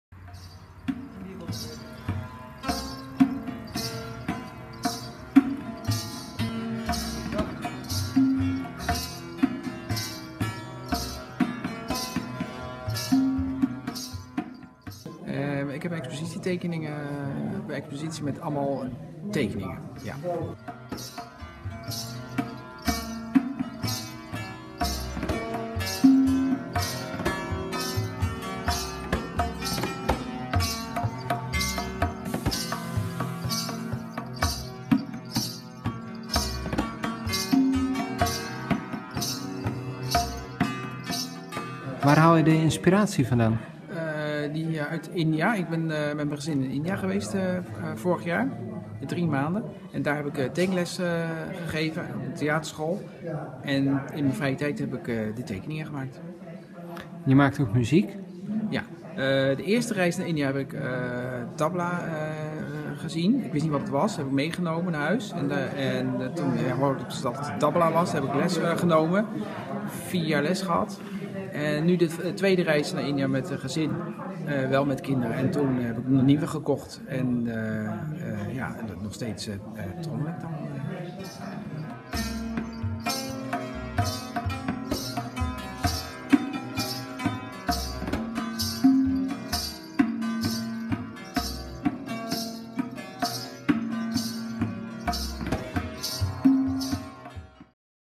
ERLEND STEINER HANGT IN DE KUNSTUITLEEN
Erlend Steiner reist met zijn gezin naar India en geeft daar tekenles en tekent er zelf ook. Die tekeningen zijn nu te zien in de Kunstuitleen.